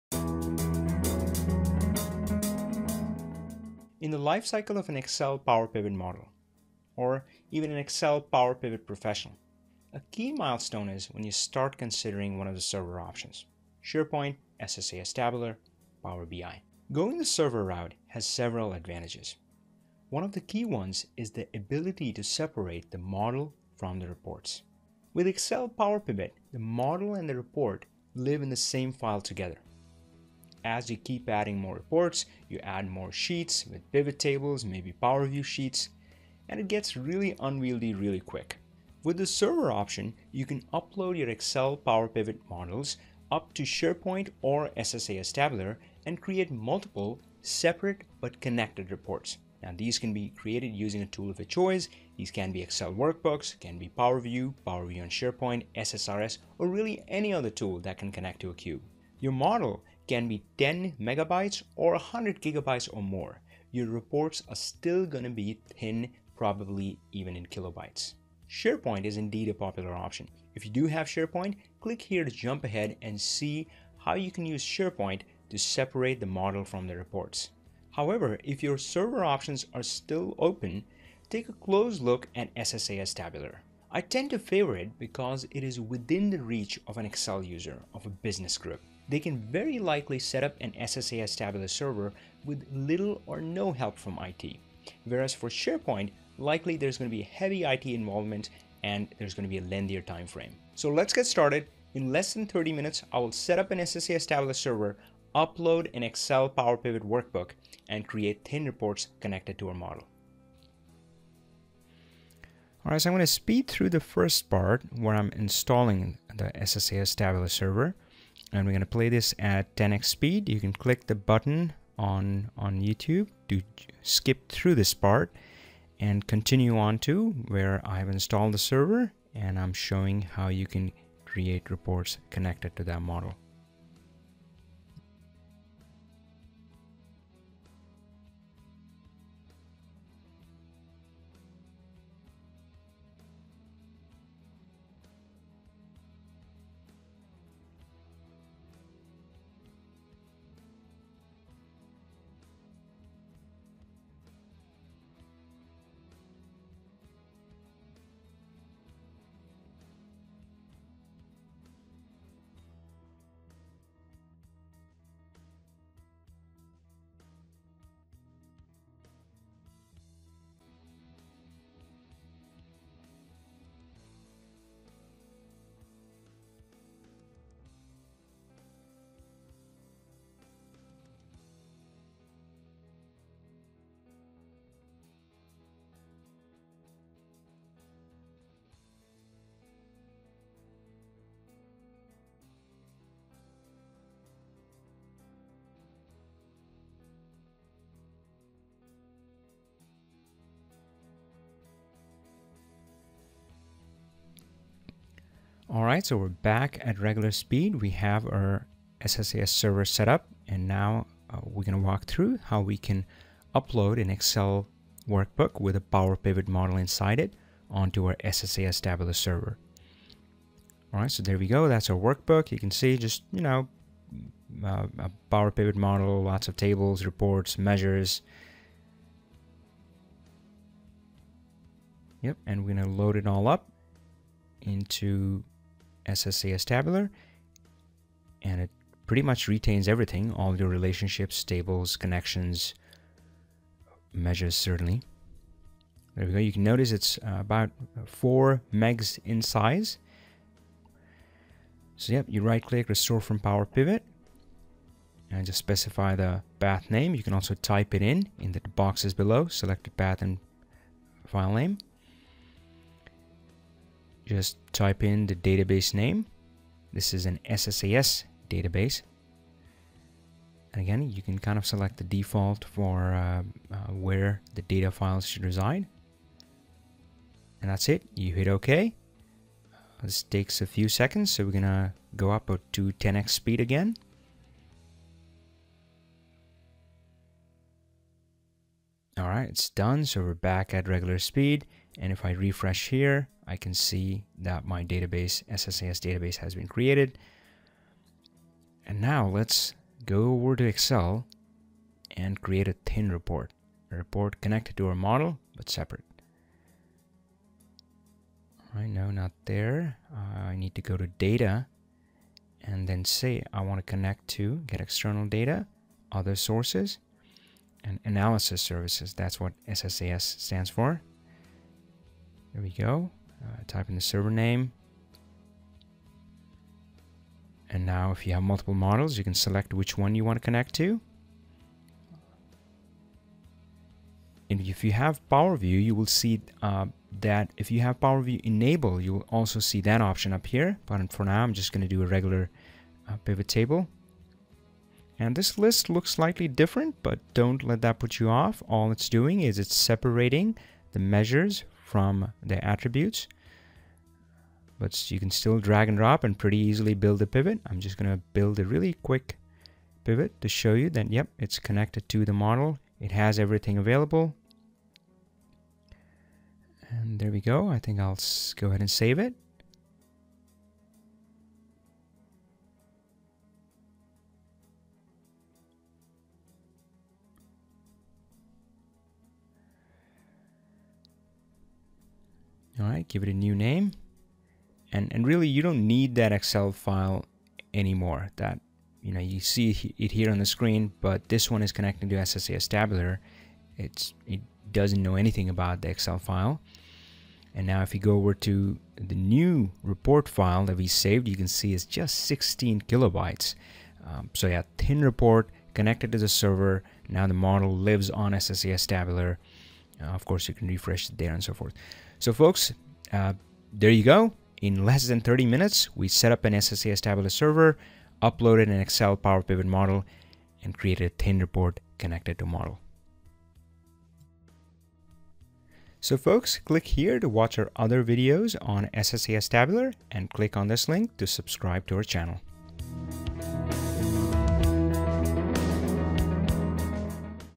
Excel Power Pivot to SSAS Tabular in less than 30 minutes
Video shows the key benefit of going the Server Route - the ability to separate the Model from the Reports. Of the available server options - SharePoint, SSAS Tabular, Power BI - it focuses on SSAS Tabular, which is relatively easy to setup.